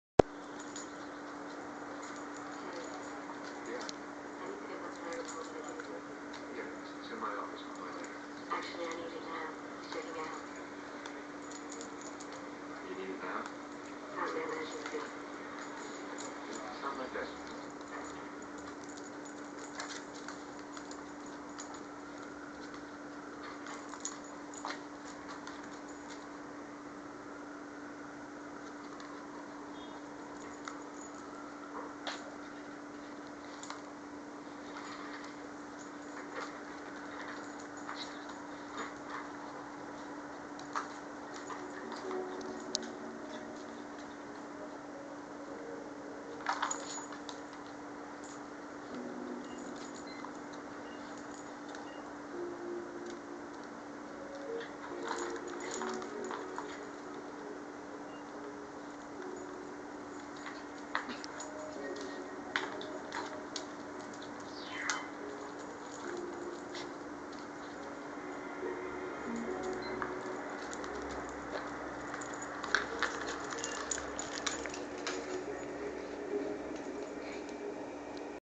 Lola wrestling Pogi
3lb maltese tries to wrestle a 20lb shih tzu